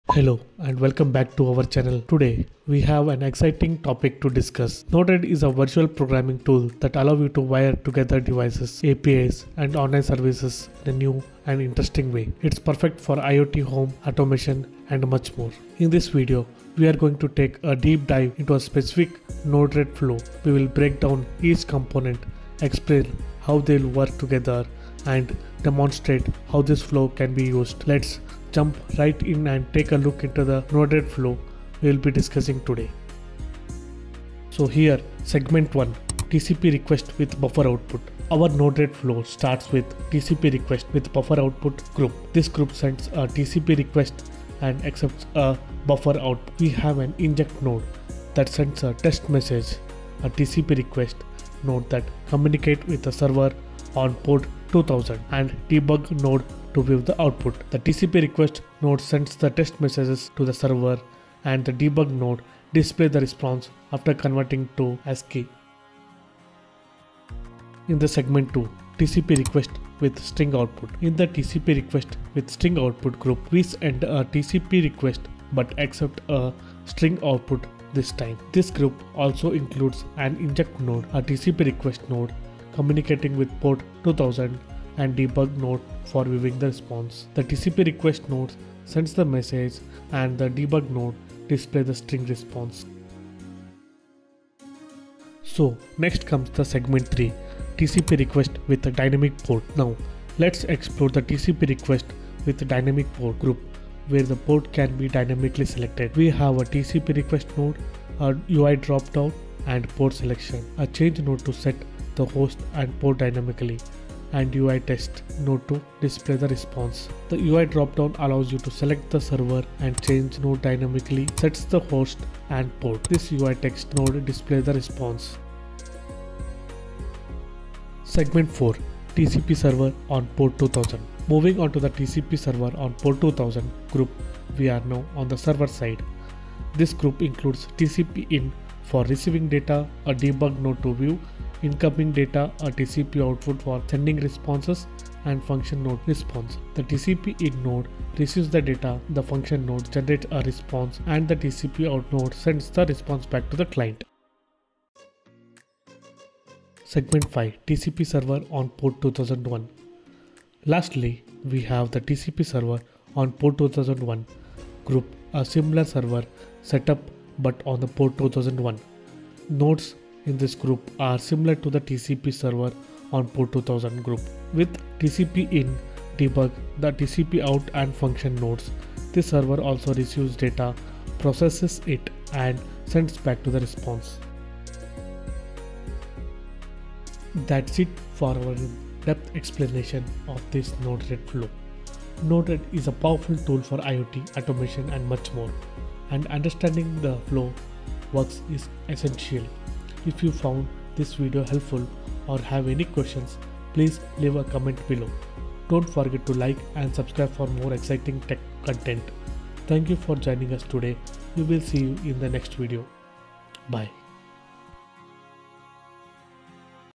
Demystifying Node-RED: Exploring a Complex Flow Step-by-Step
node red
node red tutorial
node-red

Welcome to our Node-RED tutorial! In this video, we dive deep into a complex Node-RED flow, dissecting it node by node to help you understand how this versatile tool works.

Learn how to send and receive TCP requests, convert data, and set dynamic ports for flexible communication. We also explore the server-side, demonstrating how to create two distinct servers that interact with clients.

Node-RED is a powerful visual programming tool used in IoT, automation, and more, and this breakdown will give you valuable insights into its capabilities.

If you're of IoT and automation, this tutorial is a must-watch. Have questions or suggestions?

Thank you for joining us on this Node-RED journey.